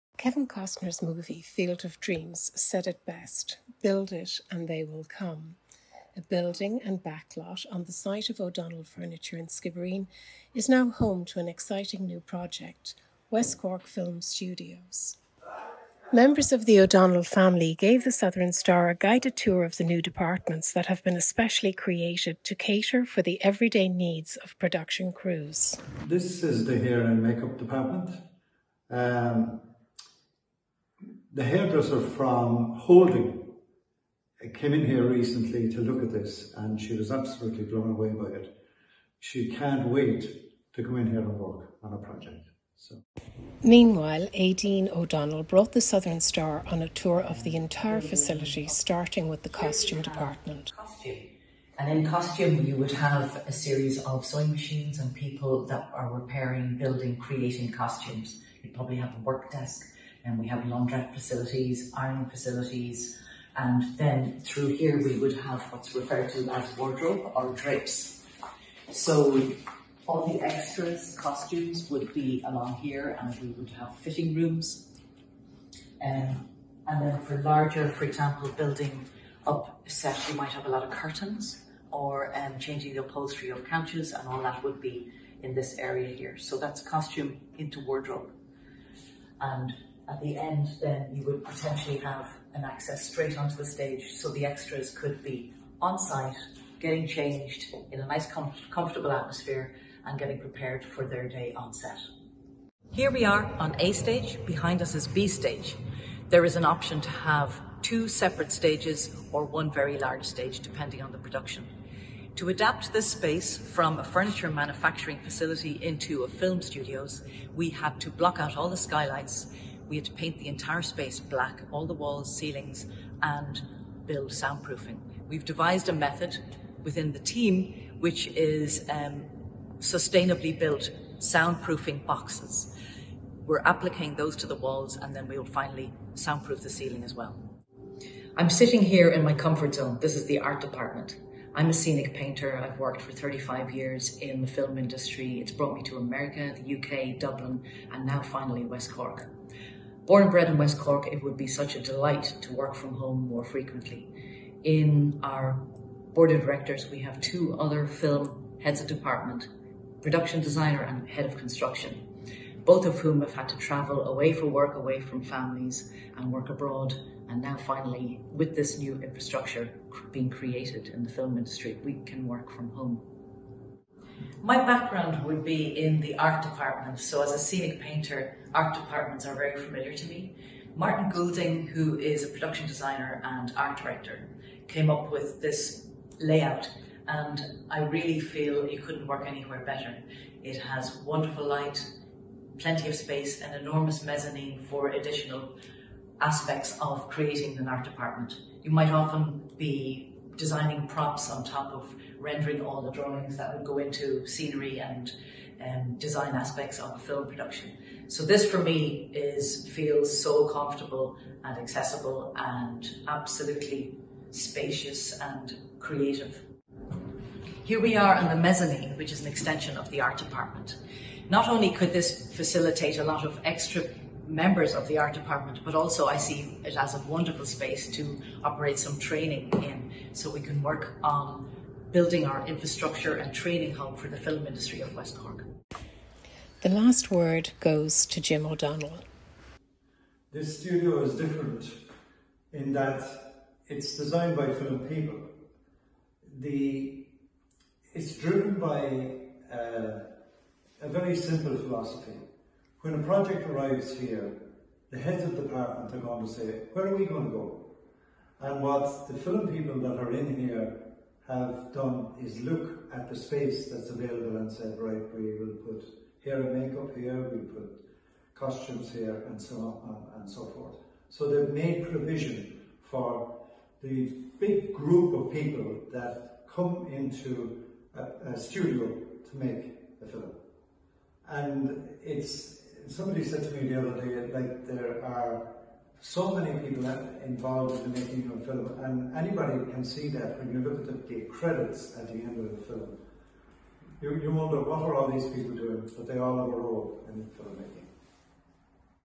Behind the scenes at West Cork's new film studios
Jackie Keogh of The Southern Star went behind the scenes at the West Cork Film Studios to get a glimpse into why the owners are positive it will help make the region a destination for filming in Ireland.